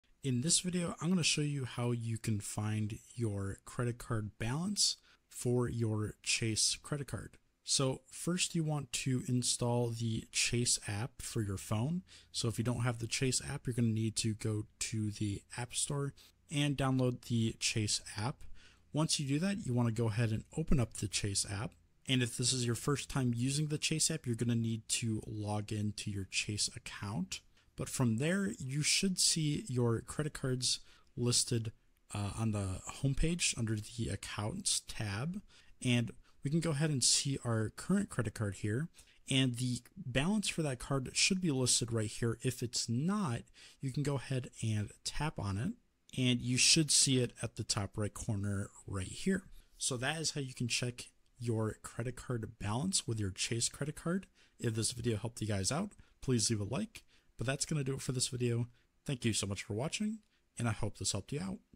How To Check Chase Credit Card Balance
Learn how to check or view your chase credit card balance amount in this step by step tutorial.